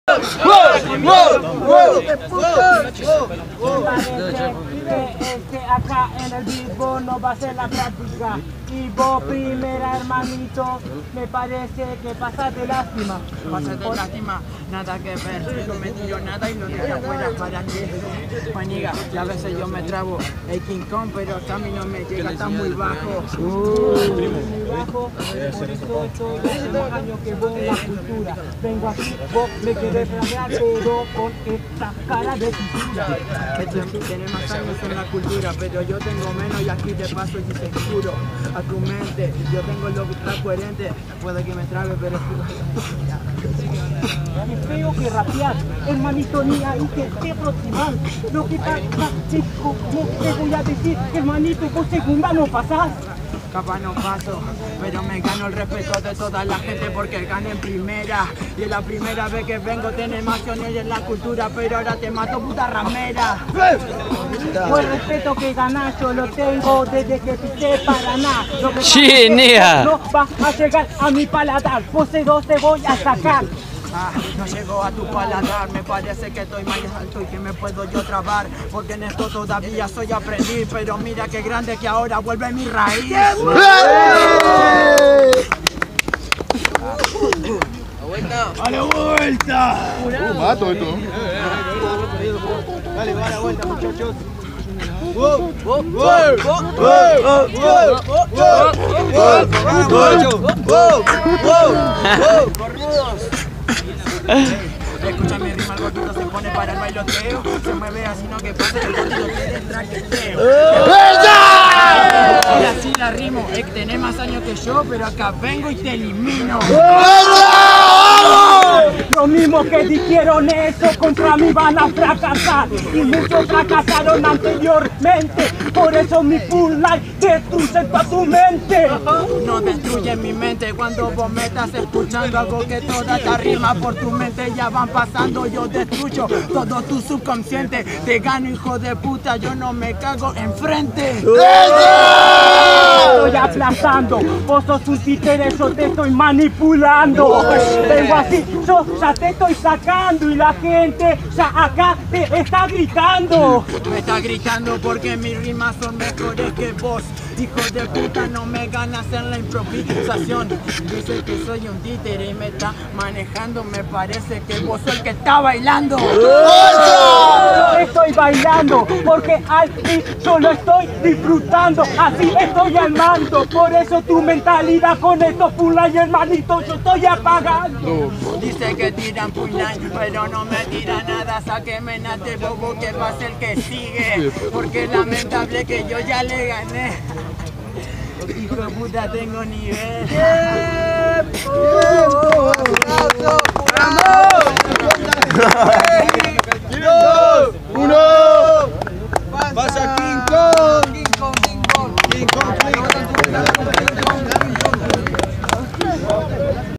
Emaa vs King Kong 4to Warrior del Under 3era Edicion
Competencia: Warrior del Under

Modalidad: 1 vs 1 (4 x 4)

Lugar: Parana, Entre Rio - Plaza de mayo

Ganador: King Kong